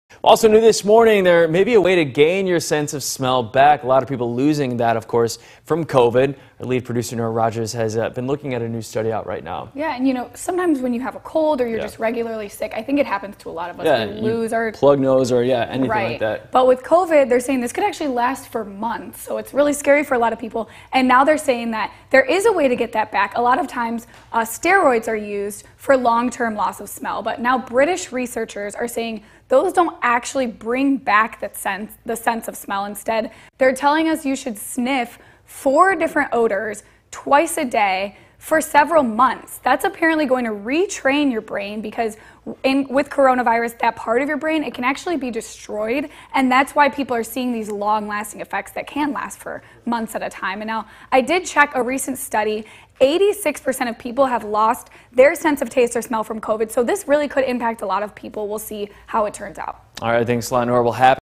Re-training your sense of smell
A British study says there may be a new way to bring back your sense of smell after losing it from covid-19.